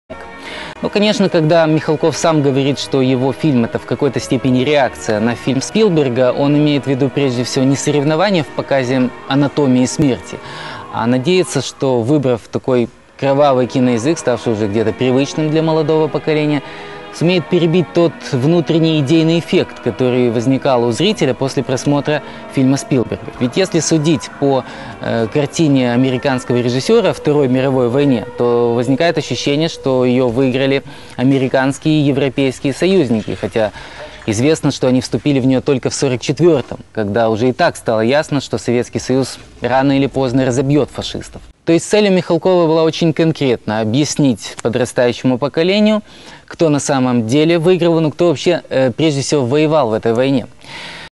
Правда-матка о войне от канала АТВ
Давно уже стоило наглядно показать, что же это такое - одесский пропутинский (не совсем правильно, все-таки, называть его пророссийским) телеканал АТВ. Основные передачи АТВ - политические, но вот короткий фрагмент их НЕПОЛИТИЧЕСКОЙ передачи, многомесячной давности 2010 года. Слушайте внимательно о том, когда же, по версии АТВ вступили в войну Америка и европейские союзники. Глубоко убежден, что ТАКУЮ абсолютную ложь (с нехитро продуманным путем к отступлению - свалять в случае чего дурочку и сослаться на высадку 1944 года гигантской группировки союзных военных в Нормандии), ТАКУЮ порцию идеологического яда - нельзя оправдать н и ч е м. Говорить уж наконец о той войне правду, либо же быть достойным презрения лжецом - если выбор имеется, то он простой и именно таков.